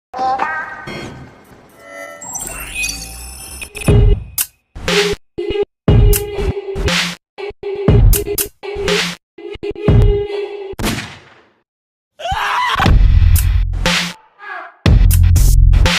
That was a very poor acting 😂
a drama in an AFF Cup semi final second leg between Vietnam vs Thailand